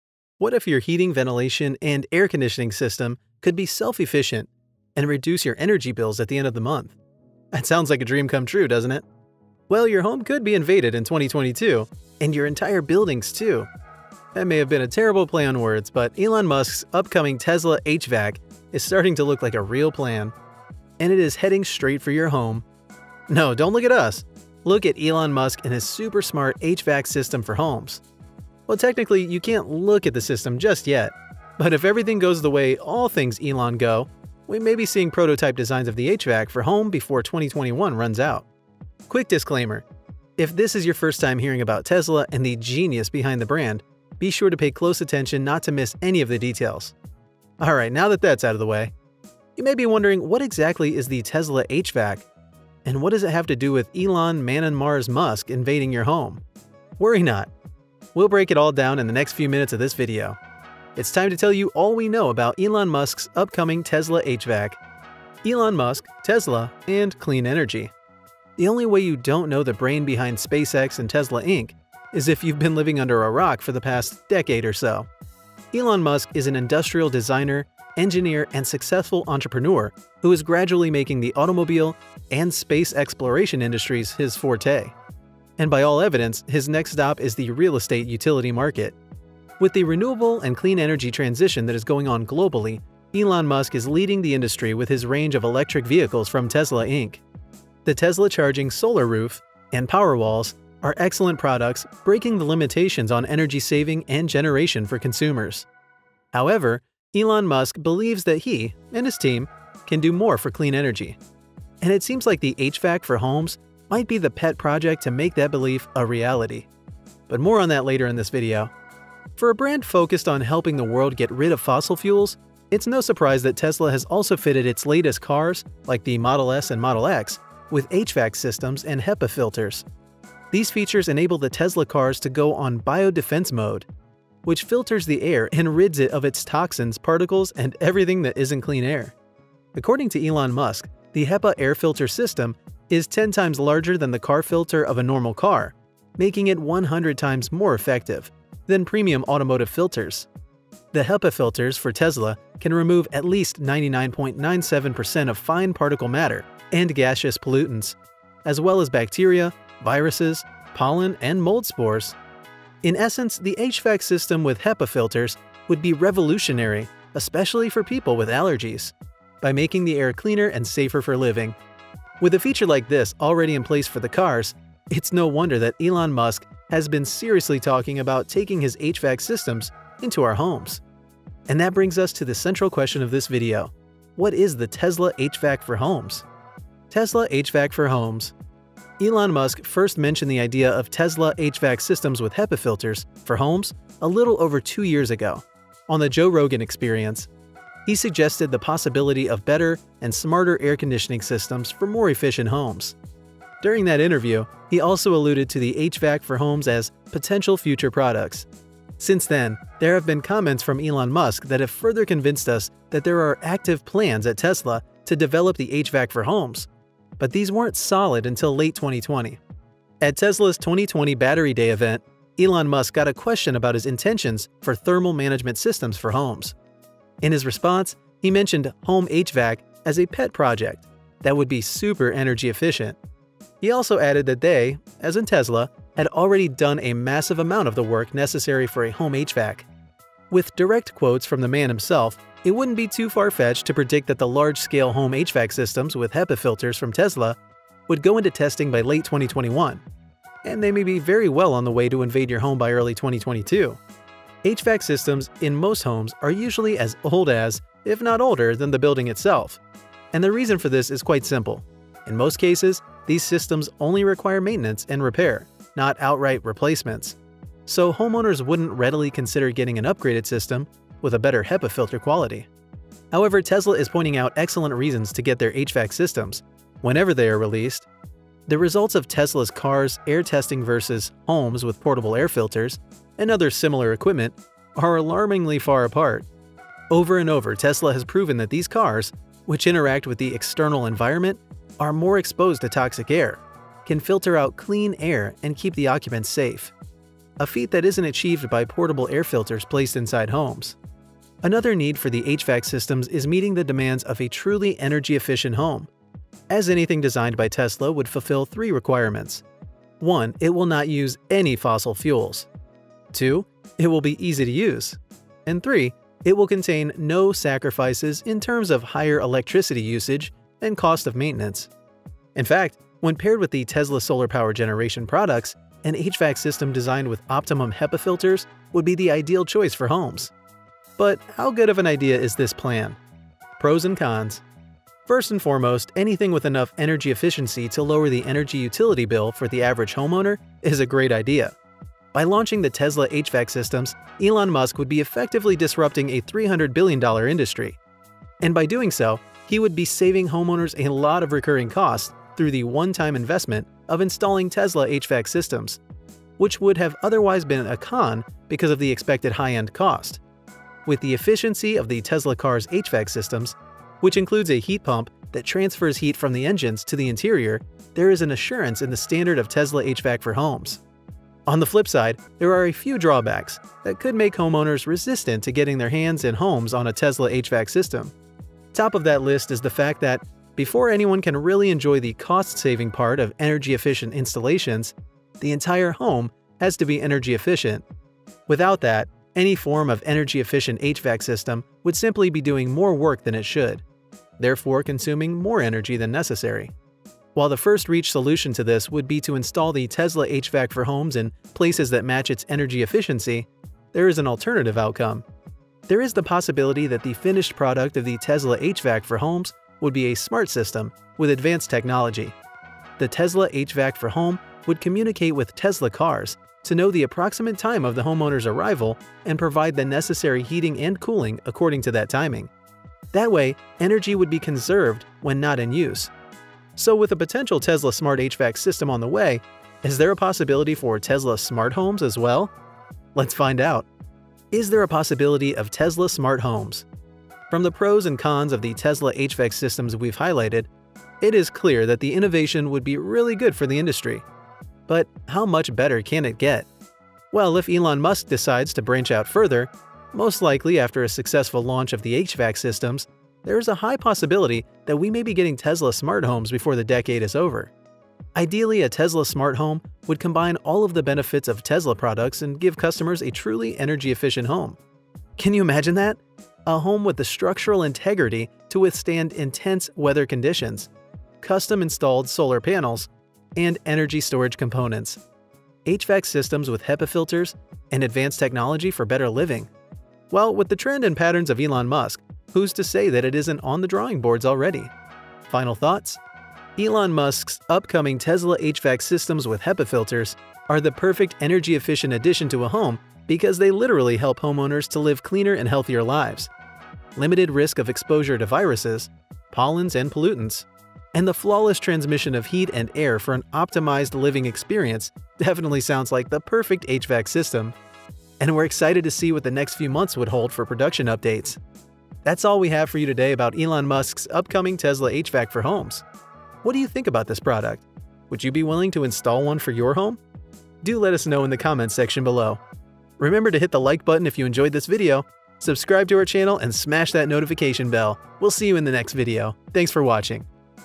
THIS Tesla HVAC System Will Change The WHOLE Industry
What if your Heating Ventilation and Air Conditioning system could be self-efficient and reduce your energy bills at the end of the month? That sounds like a dream come true, doesn't it? Well, your home could be invaded in 2022, and your entire building too!

That may have been a terrible play on words, but Elon Musk’s upcoming Tesla HVAC is starting to look like a real plan. And it is heading straight for your home. No, don’t look at us. Look at Elon Musk and his super-smart HVAC system for homes. Well, technically, you can't ¬look at the system just yet, but if everything goes the way all things Elon go, we may be seeing prototype designs of the HVAC for home before 2021 runs out.

Quick disclaimer; if this is your first time hearing about Tesla and the genius behind the brand, be sure to pay close attention not to miss any of the details. Alright, now that's out of the way.

You may be wondering, what exactly is the Tesla HVAC, and what does it have to do with Elon man-in-mars Musk invading your home? Worry not. We'll break it all down in the next few minutes of this video. It's time to tell you all we know about Elon Musk's upcoming Tesla HVAC.